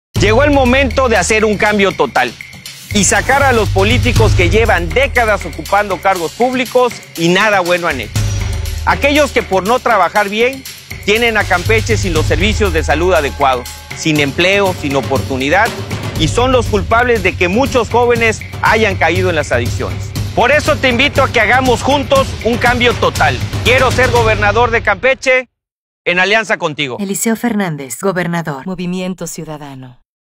Campeche va por un CambioTotal - Movimiento Ciudadano
Por fin llegó la hora de sacar a los políticos que no han hecho nada bueno por Campeche. Con Eliseo Fernández Montufar queremos hacer un CambioTotal y construir un mejor futuro para el estado con las y los campechanos. 🍊🍊